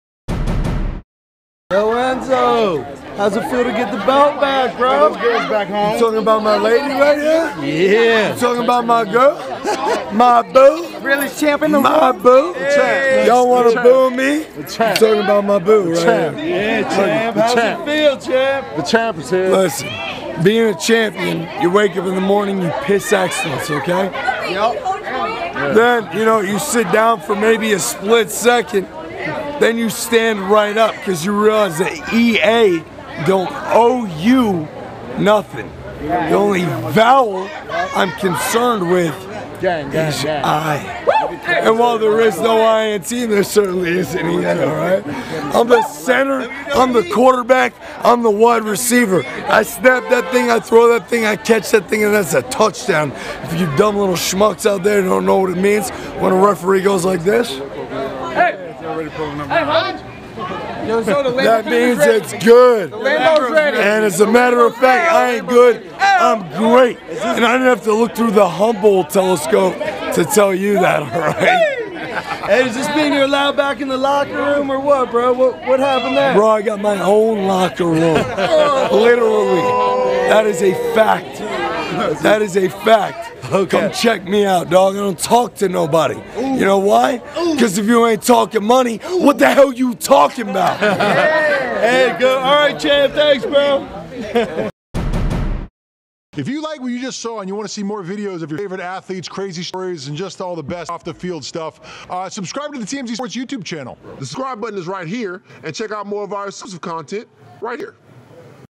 WWE's Enzo Amore: I Got My Own Locker Room, Screw Being Banned!! | TMZ Sports
WWE superstar Enzo Amore says he could care less about being banned from the WWE locker room ... 'cause he's got his own damn green room.